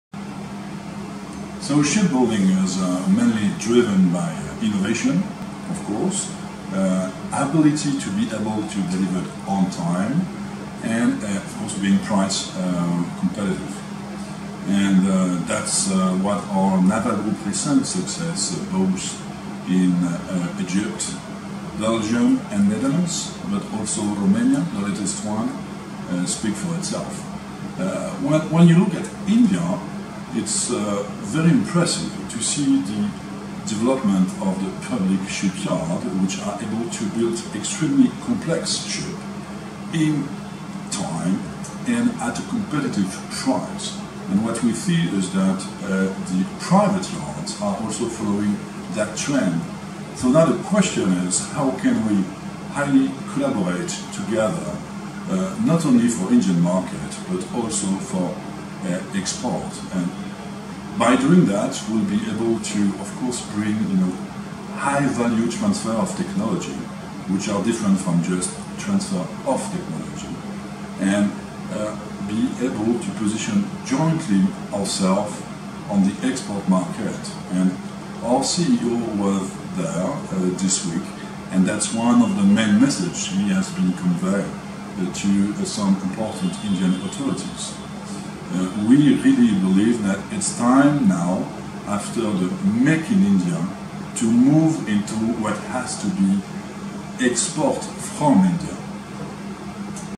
Nicolas de La Villemarque Vice President India, Asia & Pacific, Naval Group Part 2
Nicolas de La Villemarque Vice President India, Asia & Pacific, Naval Group in an exclusive with ADU at International Seminar on Nation Building Through Shipbuilding 2019. Part 2